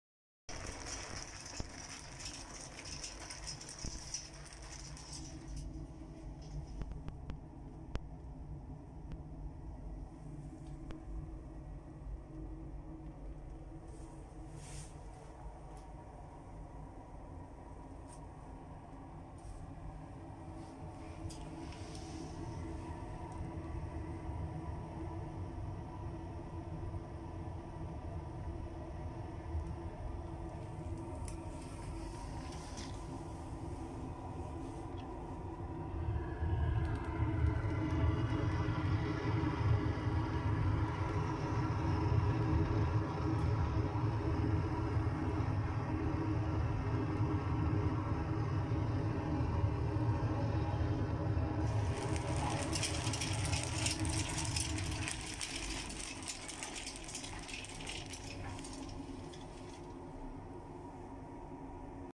new model BMAX pellet burner and combi boiler ( wood - pellet )
Combi boilers B-MAX can work with Pellet and wood , in this video we can see the new model of BMAX Pellet BURNER, this model was EBBR03 and have inside in package one WiFi modem, after you download the app form this

you can pair your Heat modem and you can control your burner directly from you smartphone, also your service can set up the burner remotely and they can help you faster than before.

enjoy your new BMAX BURNER!